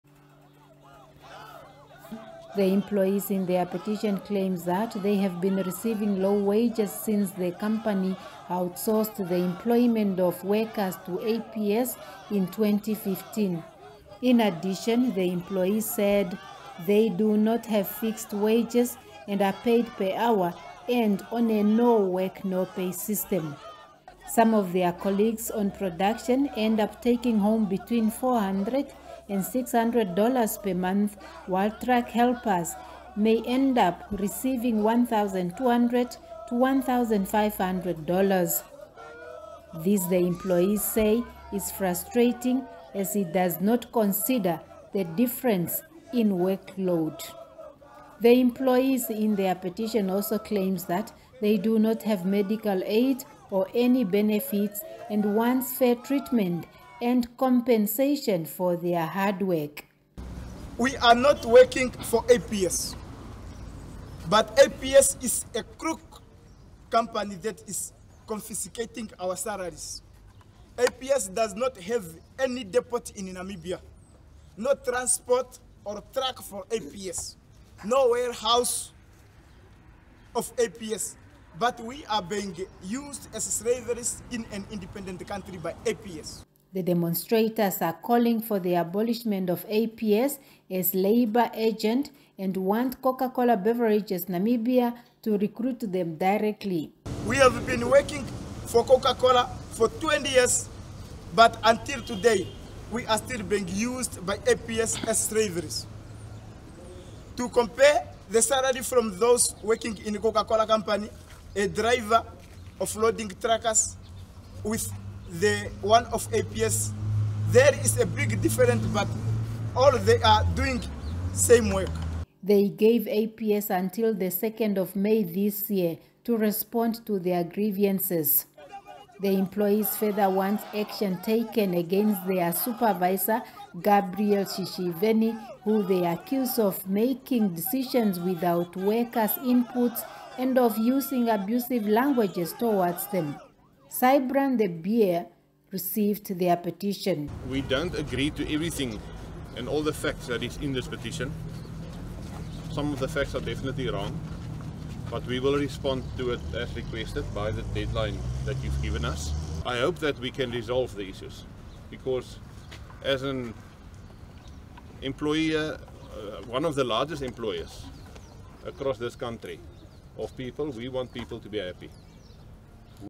Coca-Cola's Oshakati branch employees stage demonstration against APS - nbc
Employees of Coca-Cola's Oshakati branch staged a demonstration in protest against Africa Personnel Service (APS), the biggest labor hire company in Namibia.